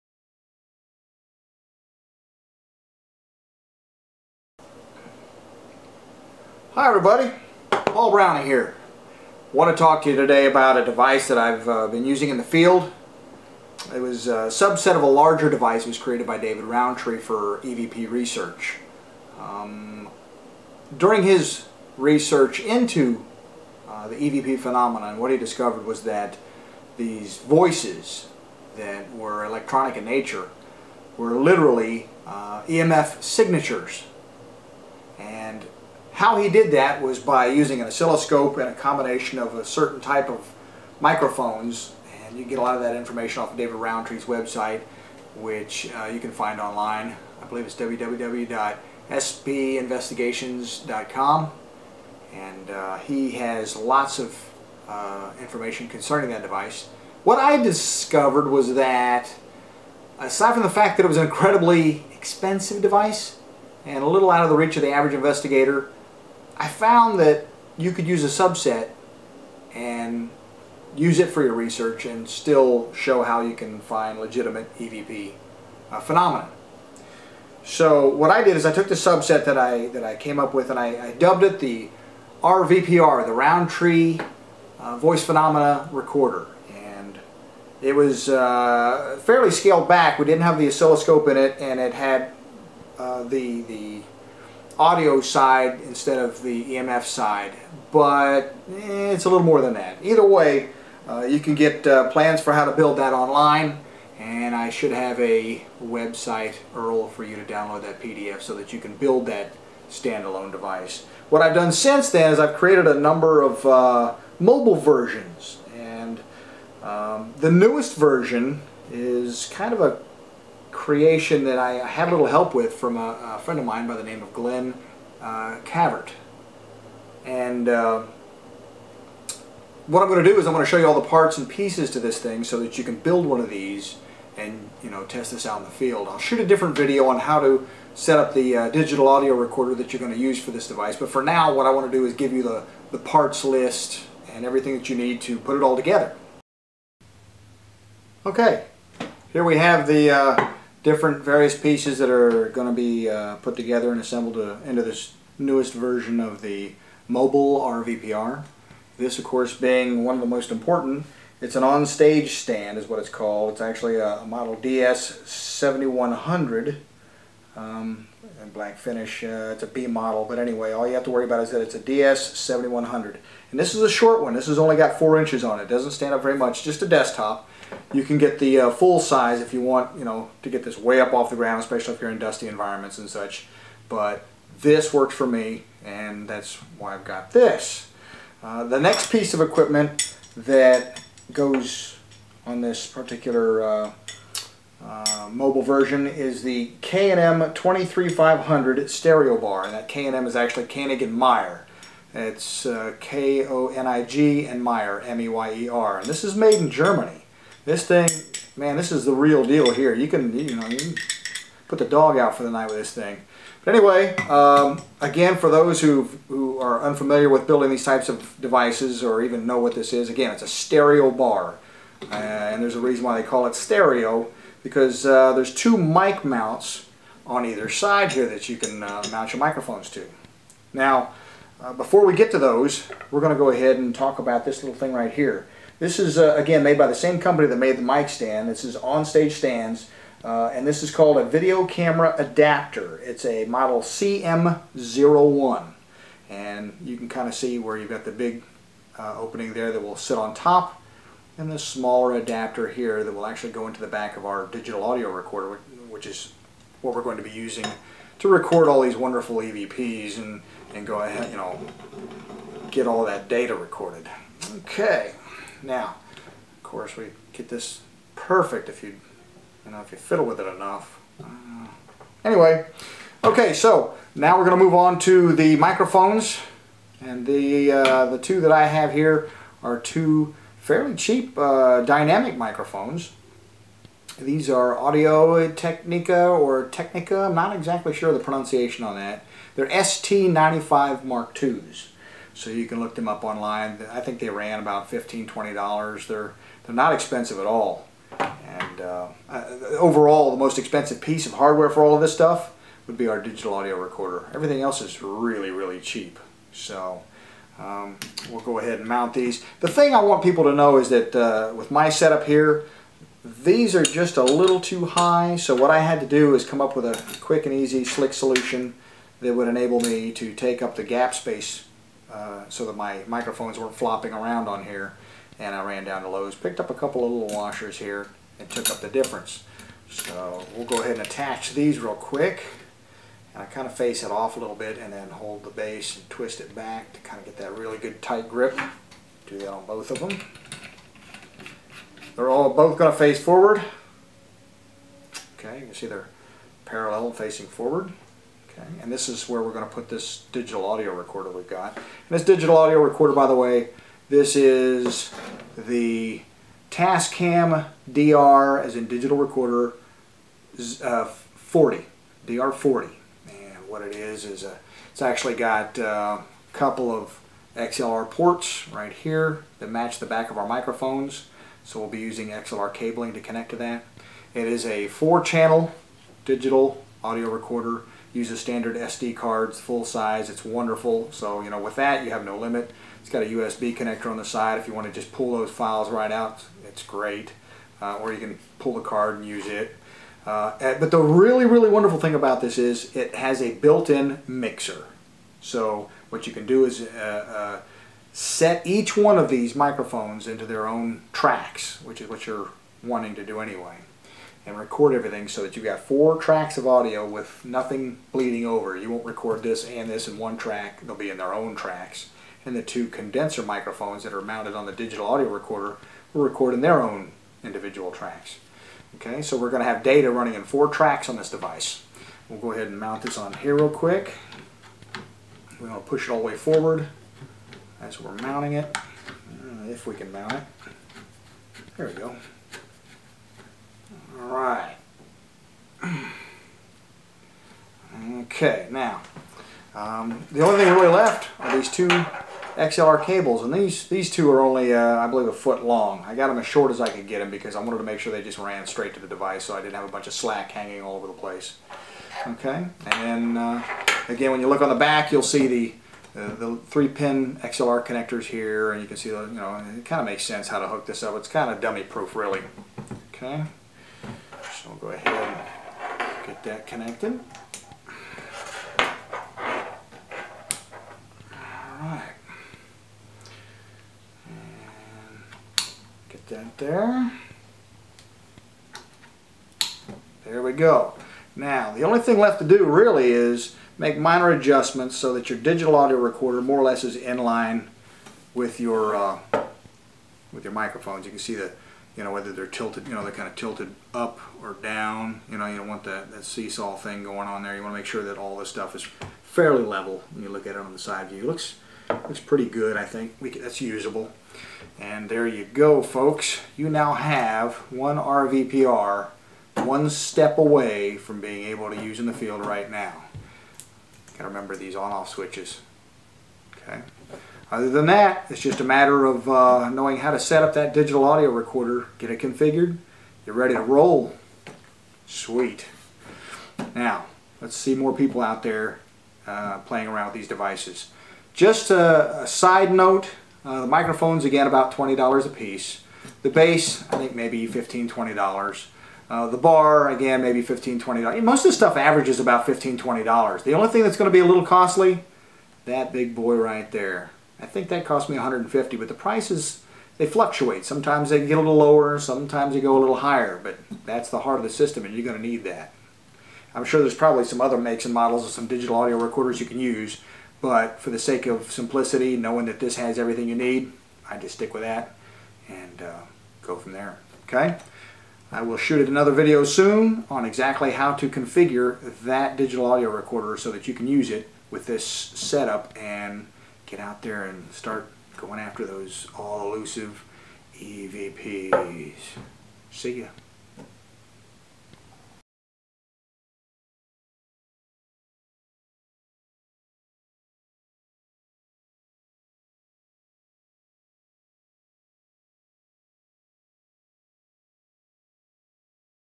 Mobile RVPR Mark II (Parts and Assembly)
List of parts and assembly shown for the RVPR Mark II.  The RVPR (Rountree Voice Phenomena Recorder) is used to capture EVP (Electronic Voice Phenomenon).